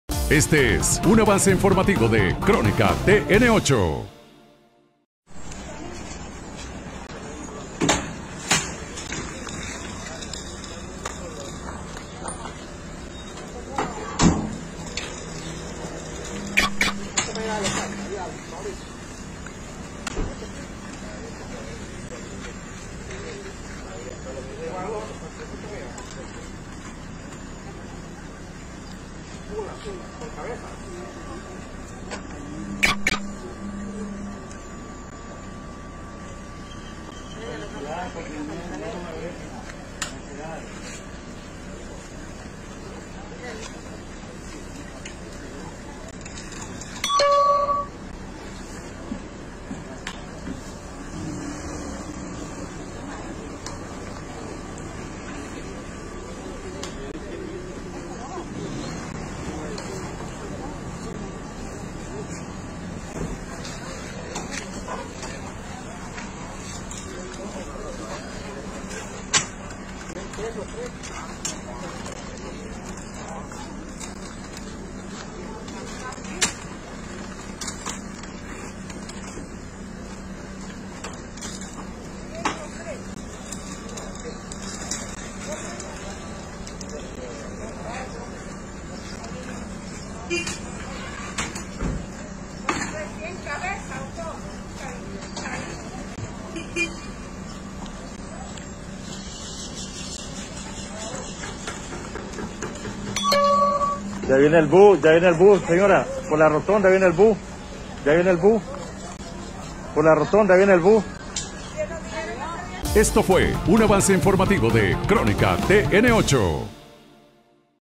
Avance Informativo: Mujer da a luz en un bus de Chinandega - Nicaragua
Se hizo viral en redes sociales de Nicaragua, el momento que una mujer dio a luz tras abordar un bus en la terminal de buses en Chinandega, el cual afortunadamente nació sin complicaciones.